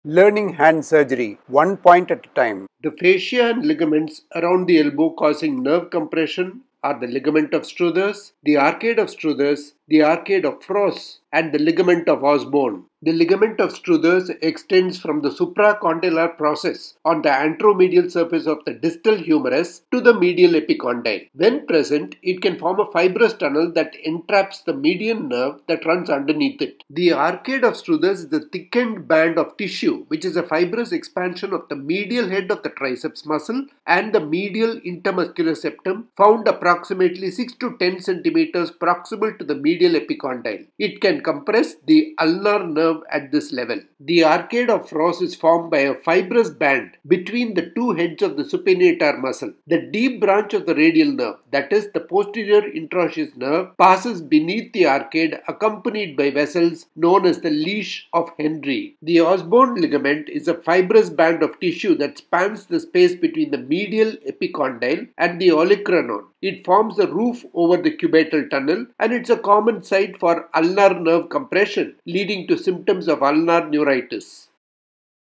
Shorts # 175: Fascia and Ligaments around the elbow that can cause Nerve Compression!
This short video is to clarify the different fascia and ligaments around the elbow that can cause nerve compression!